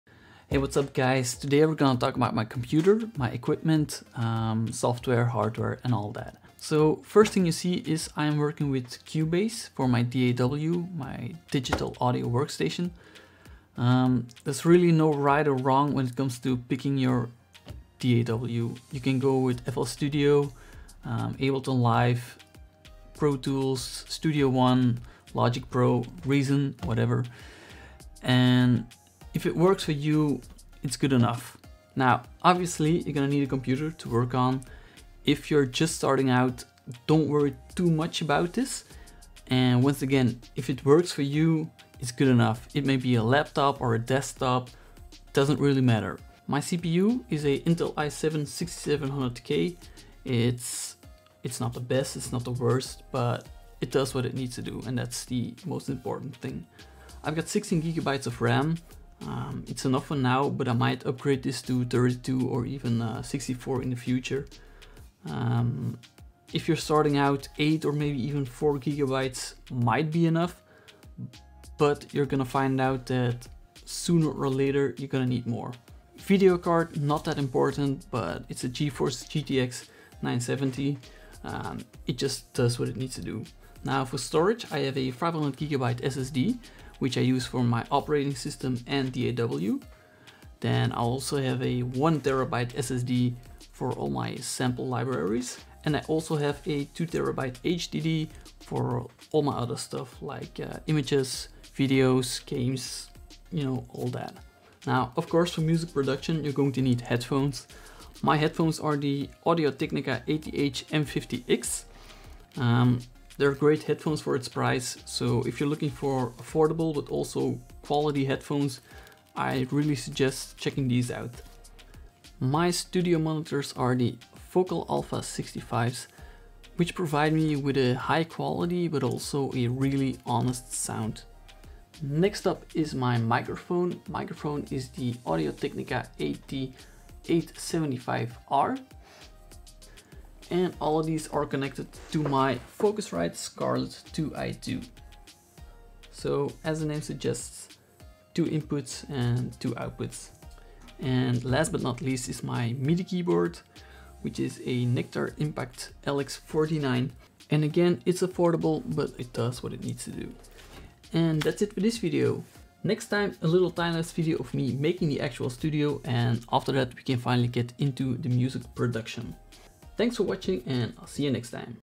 MY STUDIO EQUIPMENT
In this video I'm going through all the hardware in my "studio", and also which DAW (Digital Audio Workstation) I'm using.

DAW: Cubase Pro 9.5
CPU: Intel i7 6700K
Memory: 16GB RAM
Videocard: GeForce GTX 970
1 x 500GB SSD (OS + DAW)
1 x 1TB SSD (Sample libraries)
1 x 2TB HDD (Images, videos, games...)
Headphones: Audio-Technica ATH M50x
Monitors: Focal Alpha 65
Microphone: Audio-Technica AT875R
Audio Interface: Focusrite Scarlett 2i2
MIDI keyboard: Nektar Impact lx49+

If you have any suggestions to improve the content of this channel, make sure to let me know!